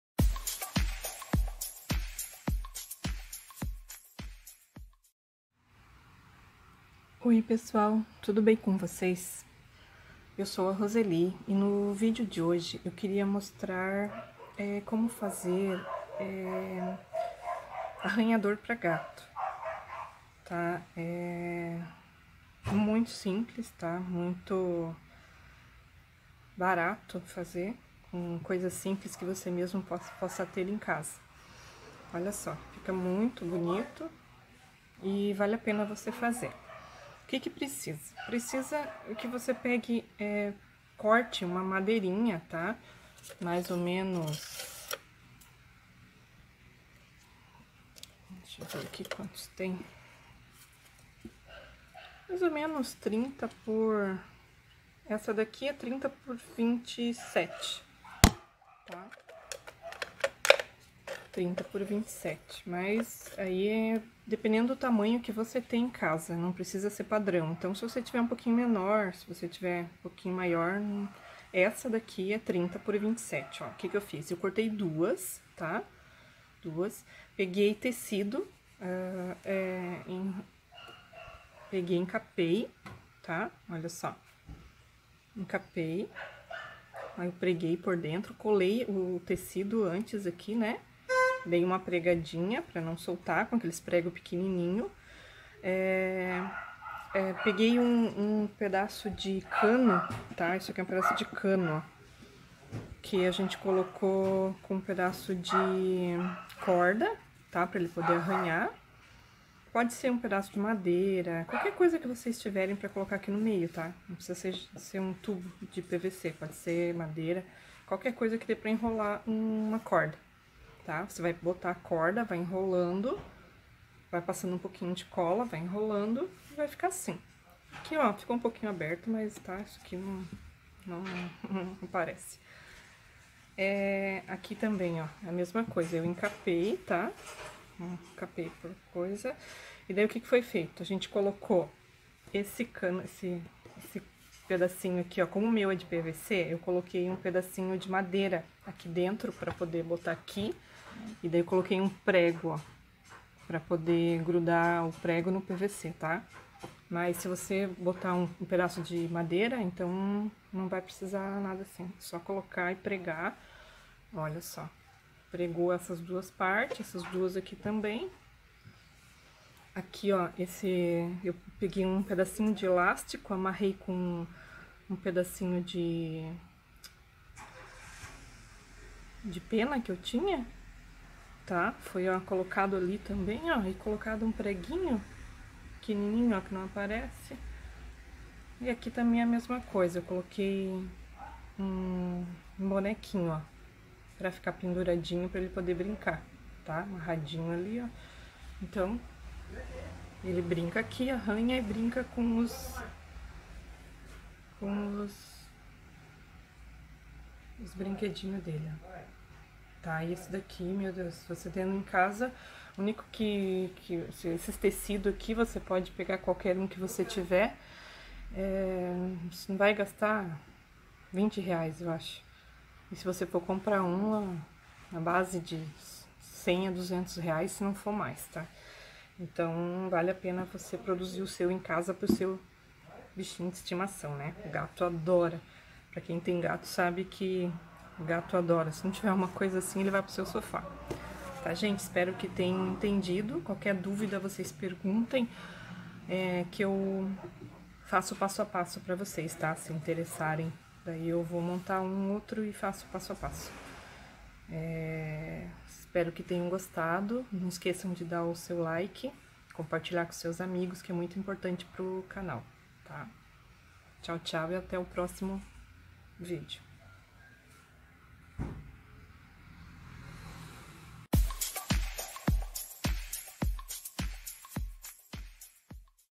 ARRANHADOR PARA GATOS - FAÇA VOCÊ MESMO
Faça você mesmo  um  lindo ARRANHADOR para seu gato, super fácil.

Vai precisar de dois pedaços de madeira quadrado, um cano de PVC ou um pedaço de madeira, fio, cola quente.
Use sua criatividade.

Dúvidas? Deixem nos comentários.